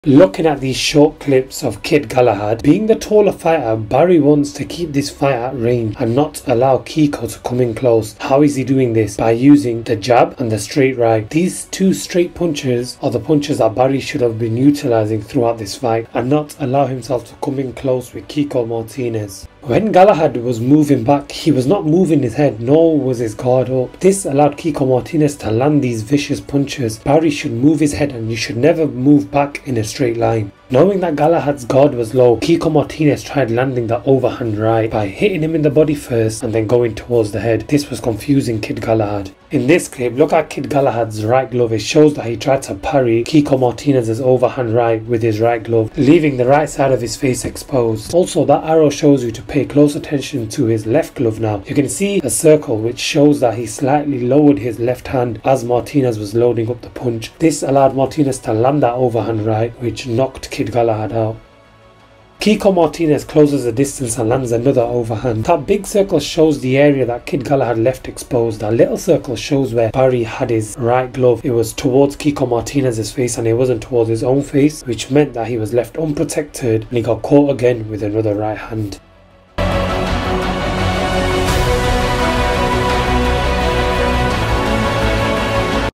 WAS THIS KNOCKOUT OF THE YEAR!? - Kid Galahad vs Kiko Martinez - The Short Analysis
The analysis of how Martinez caused one of the biggest upsets in boxing history against the homecoming former world champion Kid Galahad.